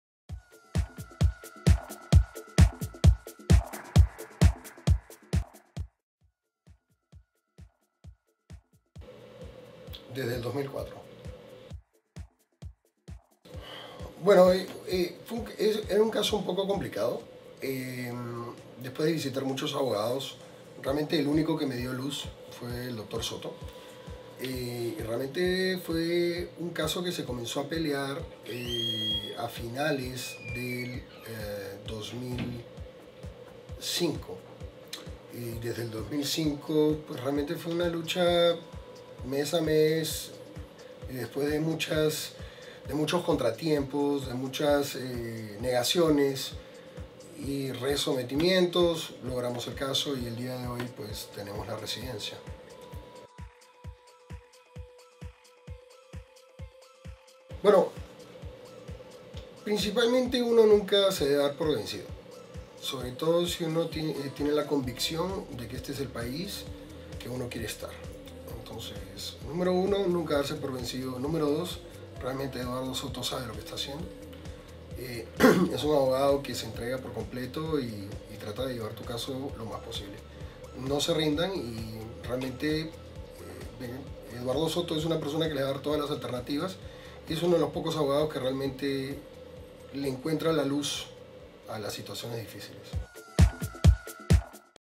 Testimonial sobre Eduardo Soto: inmigrante peruano cuenta cómo lo ayudó
En inmigración no siempre los csos son fáciles: MUCHAS VECES SON DIFICILES. Lo importante es no darse por vencido y encontrar a un abogado que vea soluciones para usted, que vea luz al final del túnel y pelee por usted.

Como este inmigrante lo dice, "si usted está convencido de que EEUU es el país en el que qeuiere vivir, no se dé por vencido".

999 Ponce De León Boulevard
Suite 1040